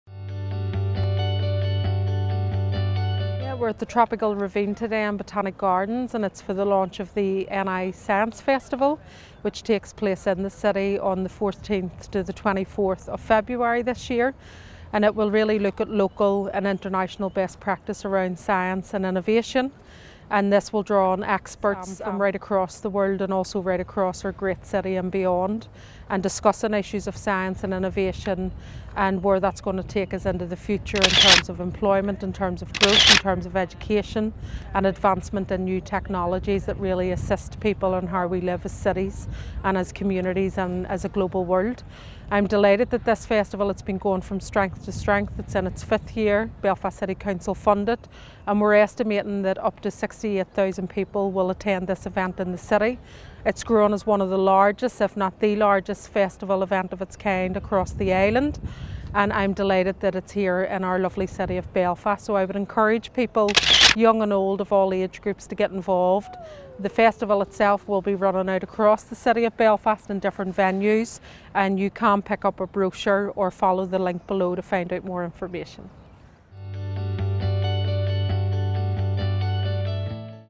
NI Science Festival launches out of this world programme for 2019
Acclaimed scientists Professor Brian Cox and Liz Bonnin, UV Yoga, and a crash course into the teenage psyche are some of the hot tickets at this year’s NI Science Festival as they announce their stratospheric programme. Lord Mayor of Belfast, Councillor Deirdre Hargey was there to officially launch the festival programme. January 2019